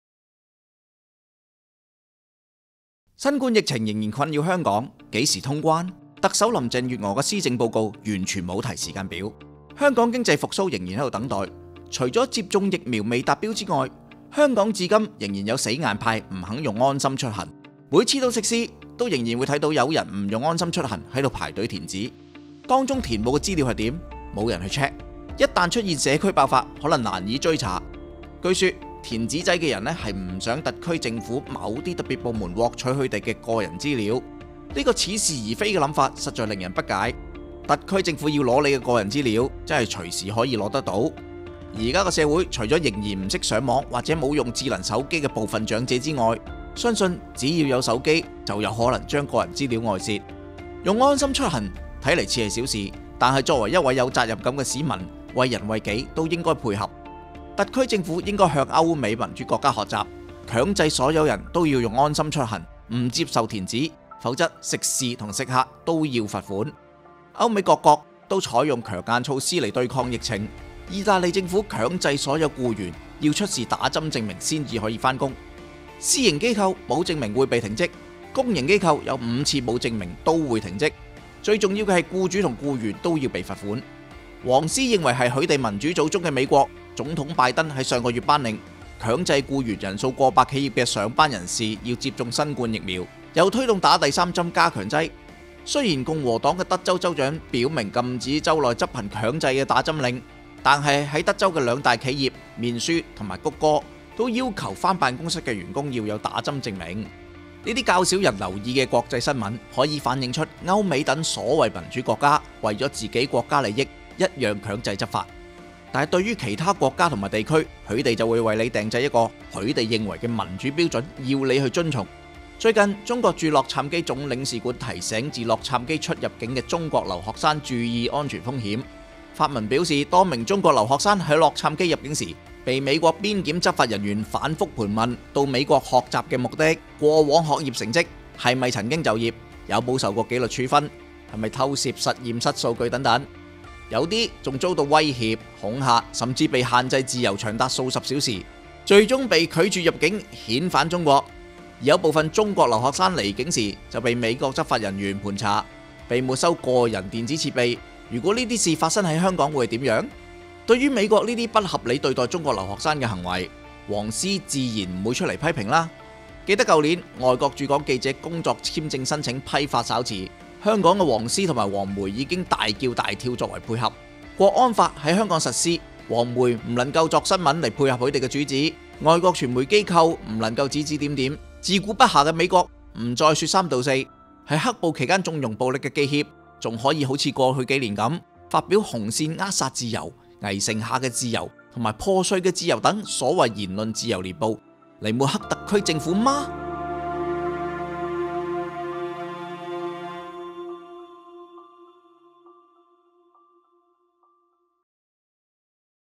聲光伴讀《美國你民我主的民主》作者：夏理仁
黃人愛以美帝的所謂民主與中國比較，對我們國家指指點點，但他們只會選擇性的拿對自己有利的來比，或只看一些表面的口講式民主，美帝強制僱員人數過百企業的上班人士要接種新冠疫苖，黃人絕口不提。美帝邊檢執法人盤查中國留學生，遣送他們回國，黃人看不到。美帝為利益而強制執行一些法例，但卻要求其他國家遵從他們訂立的所謂民主標準，黃人們視而不見。HKG報博客夏理仁的文章《美國你民我主的民主》之中，就指出美帝種種對民主雙重標準的行為，立即去片看看夏理仁怎樣說。

原片、原圖：無綫新聞片段、港台新聞片段、星島日報圖片、中國駐洛杉磯總領事館網頁截圖、記協Facebook截圖